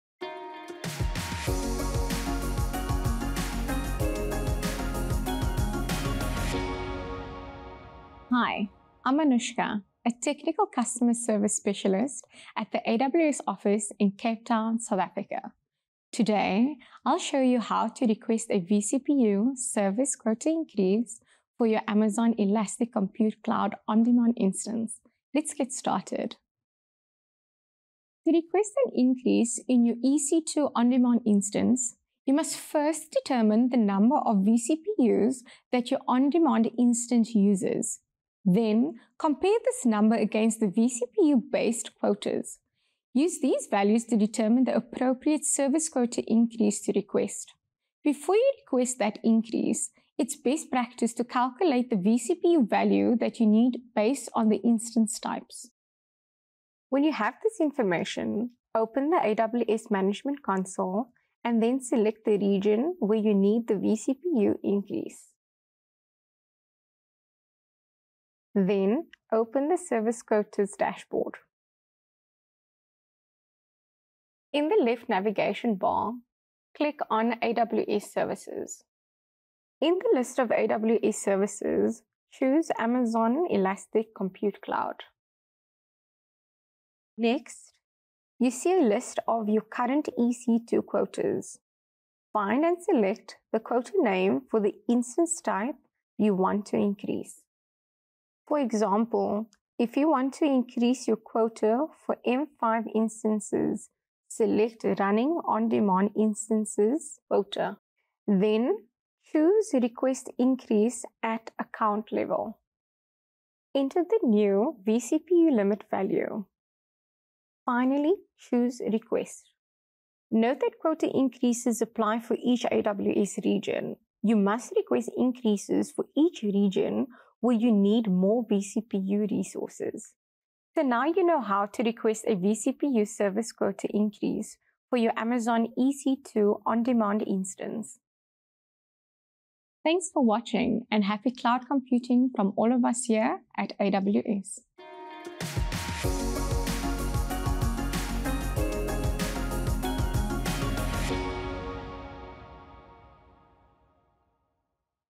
How do I request a vCPU service quota increase for my Amazon EC2 On-Demand Instance?
For more details on this topic, see the Knowledge Center article associated with this video

Anushka shows you how to request a vCPU service quota increase for my Amazon EC2 On-Demand Instance | Amazon Web Services.

0:00 Introduction
0:28 Calculate the vCPU value you need based on the instance type
0:56 Correct Region
1:16 Service Quotas via the AWS Console
1:48 Request increase at account level
2:20 Closing